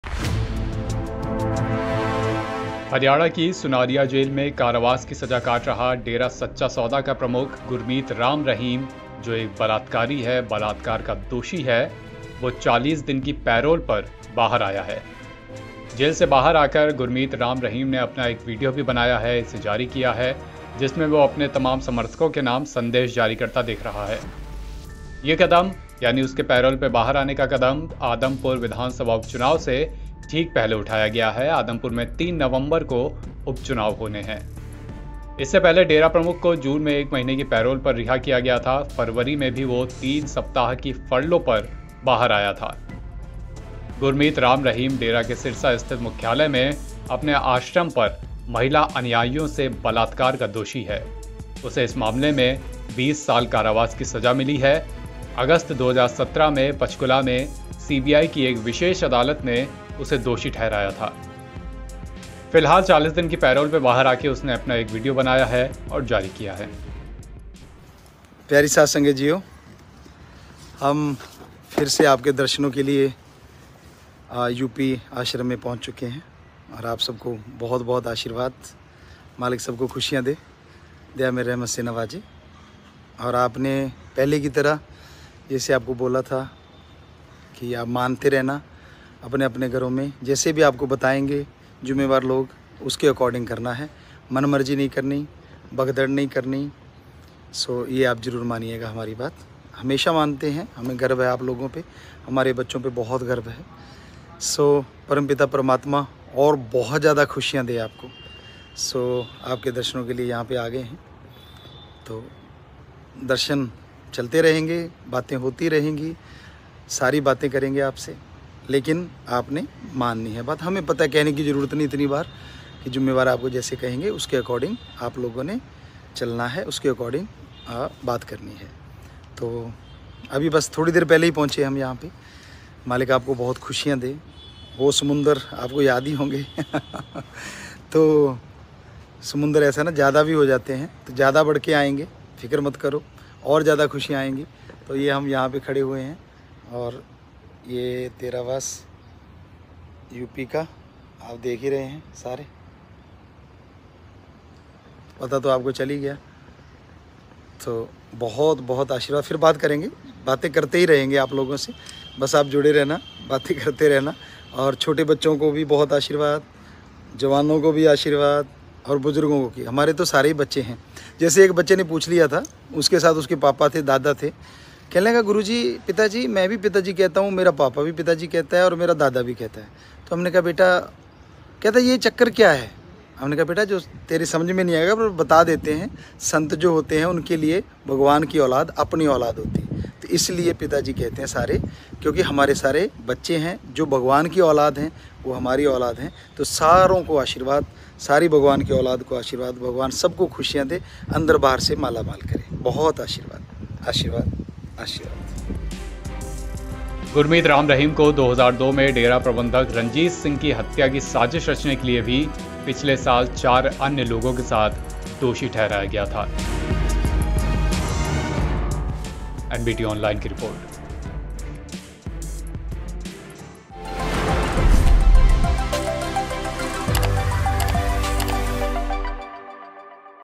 Ram Rahim Parole : 40 दिन के लिए जेल से बाहर आया है बलात्कार का दोषी Dera Chief
Ram Rahim Parole : Dera Chief out of jail for 40 days on Parole released a video immediately.